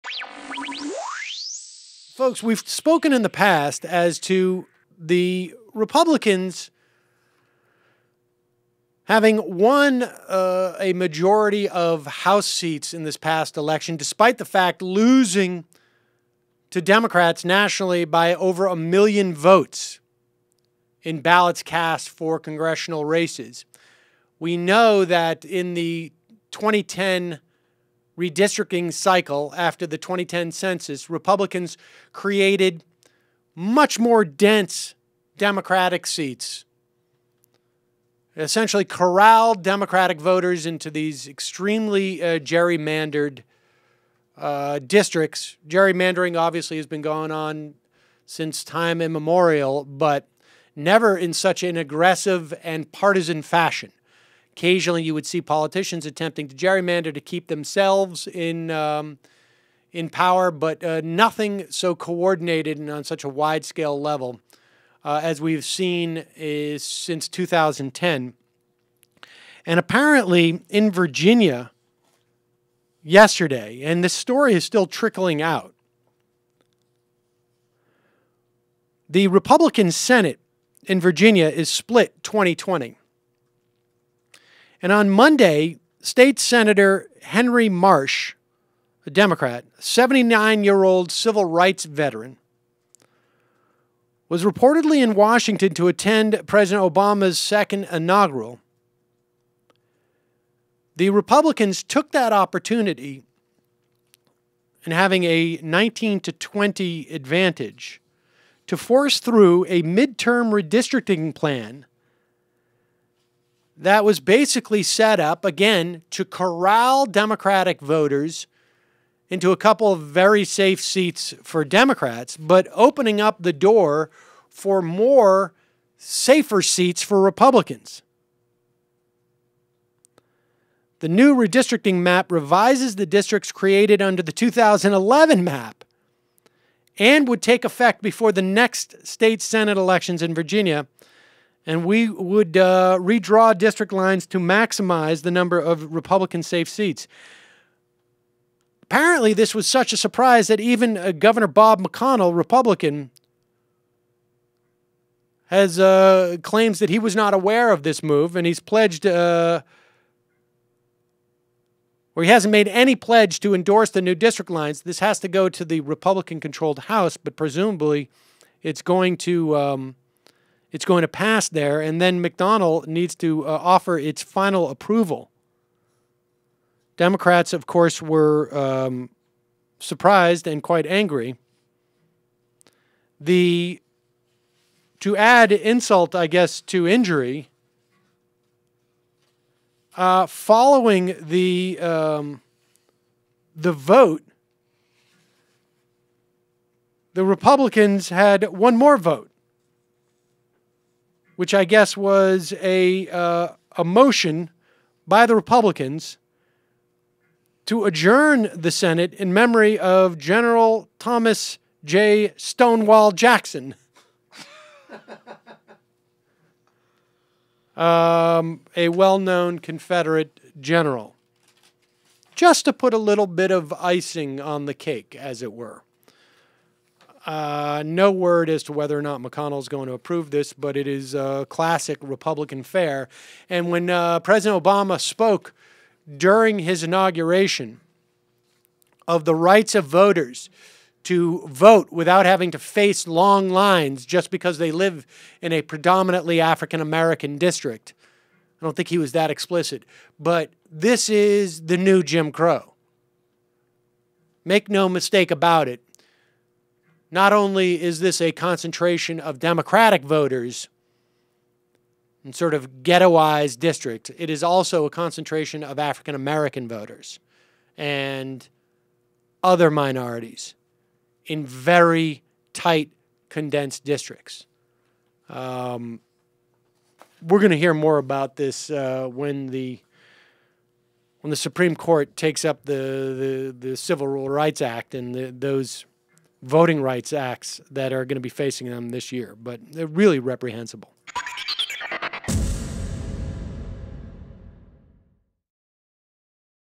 GOP Commemorates MLK Day By Disenfranchising Voters
In a move that stunned even Republican Virginia Governor McDonnell, Virginia Republicans have used President Obama's Inauguration day to make a stunningly cynical voter disenfranchisement bill law...